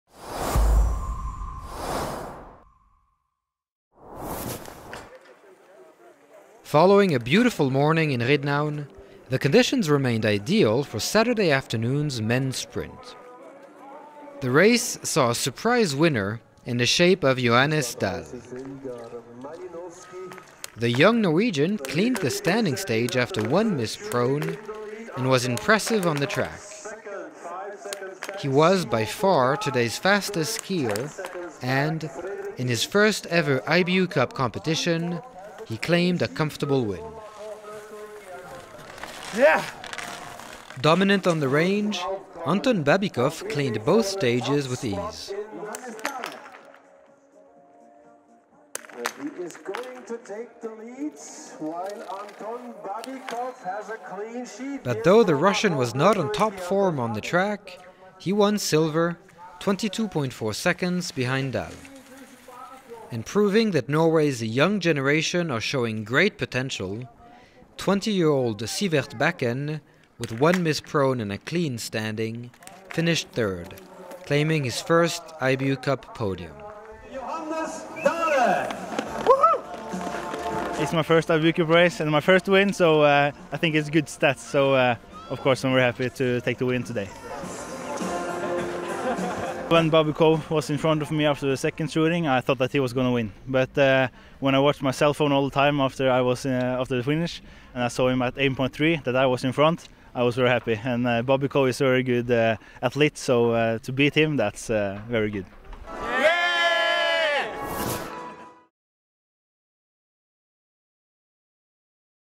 2018/19 IBU Cup 2 - Men Sprint
Highlights from the Men Sprint in Ridnaun, won by Norwegian newcomer Johannes Dale